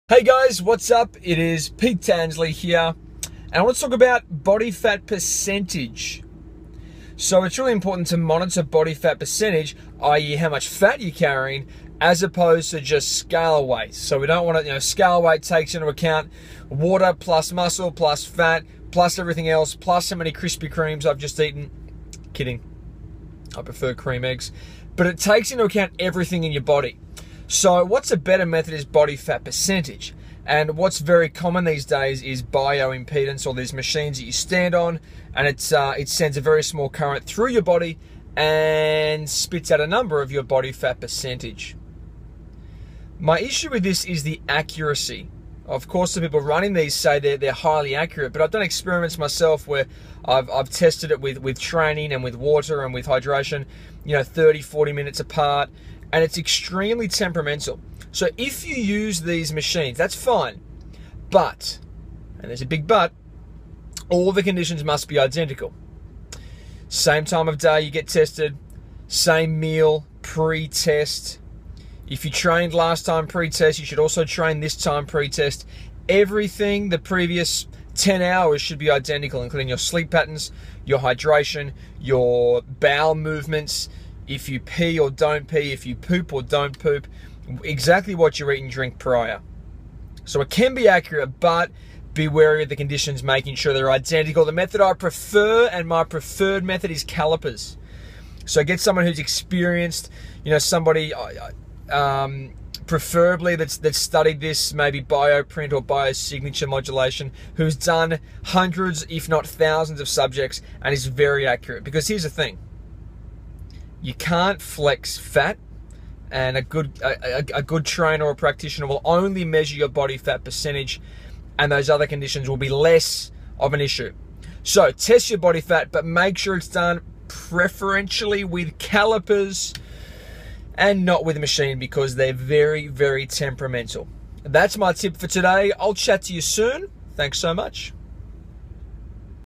Body Fat Percentage
Pete Tansley looks at the best way to measure your body fat percentage.

Is it:
- Old school callipers?
- Bio-impedance scales?
- Looking at photos?